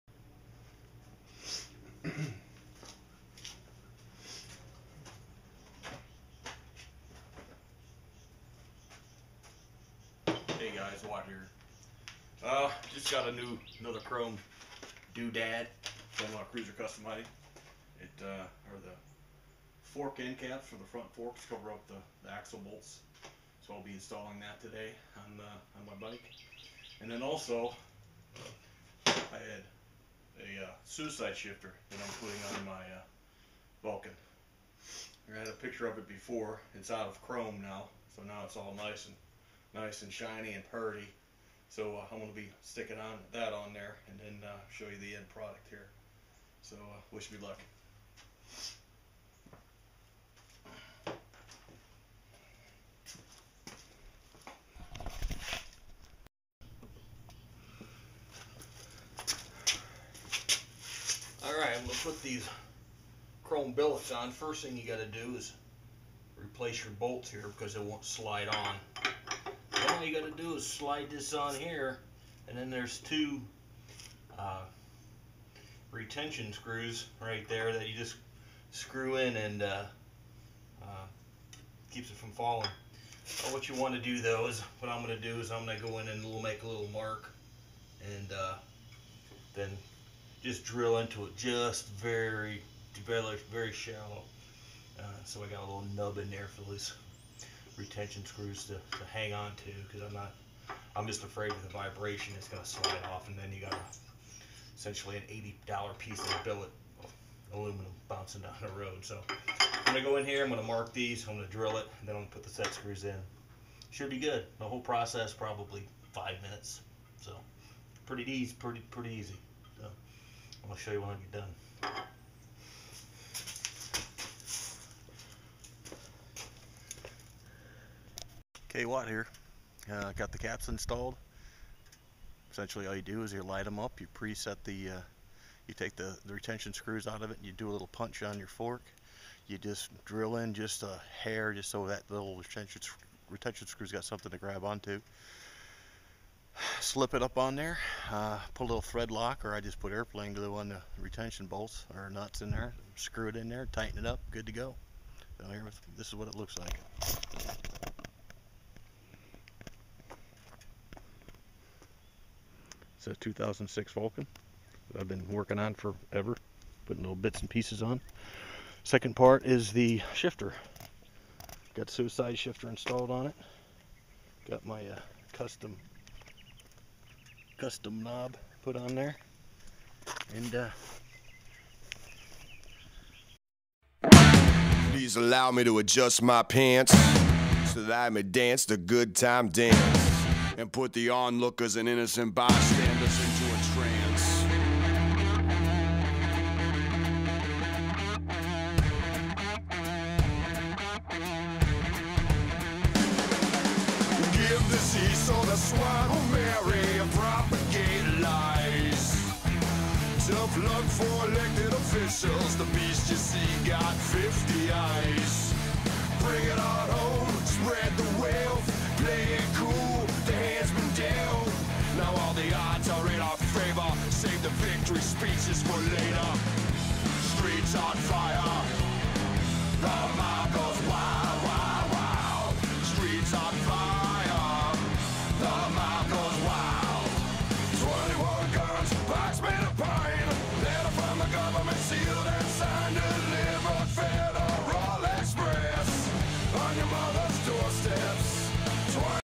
shifter
INstall suicide shifter and fork bullet covers